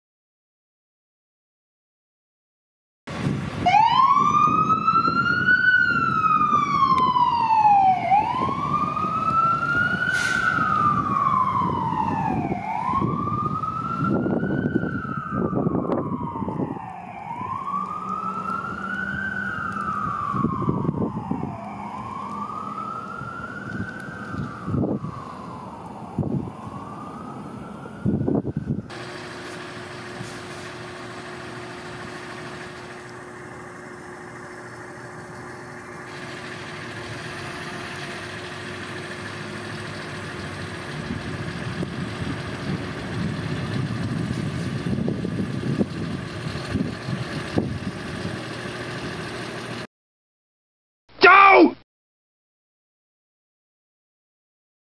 Chilliwack Engine 1 Responding + Rescue 1
Chilliwack Engine 1 responding on a snowy night. My memory card ran out just as Rescue 1 was leaving... D'oh!
27 December 2017
Chilliwack BC Canada